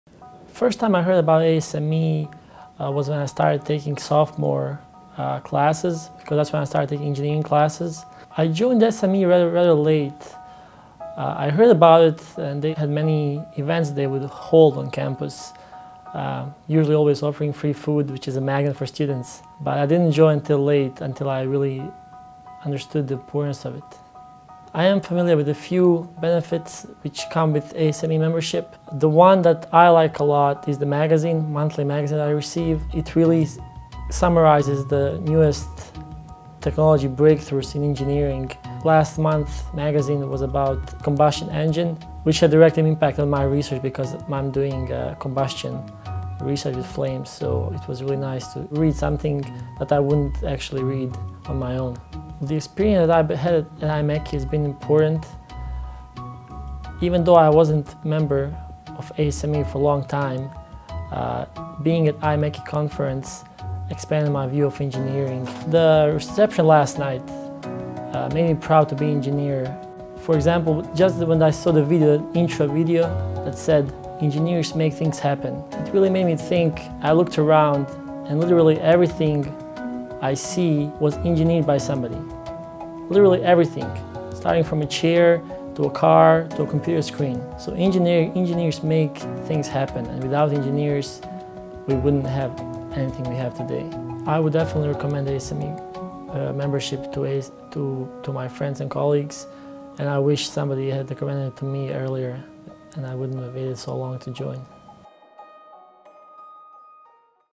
Staying Relevant
Tonci Maleta discusses how ASME has helped him keep up with the rapid pace of technological innovation in your industry.